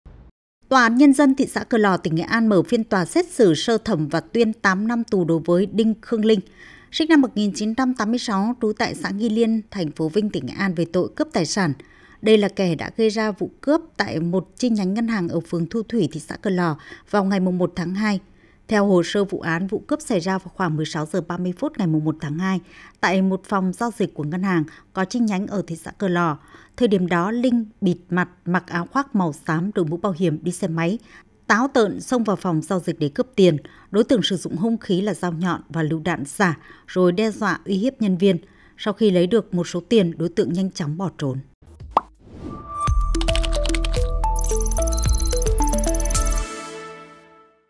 8 năm tù cho kẻ dùng lựu đạn giả cướp ngân hàng
Tòa án nhân dân thị xã Cửa Lò (Nghệ An) mở phiên tòa xét xử sơ thẩm và tuyên 8 năm tù đối với Đinh Khương Linh (SN 1986, trú xã Nghi Liên, TP Vinh, Nghệ An) về tội "Cướp tài sản". Đây là kẻ đã gây ra vụ cướp tại một chi nhánh ngân hàng ở phường Thu Thủy, thị xã Cửa Lò vào ngày 1/2/2024. ---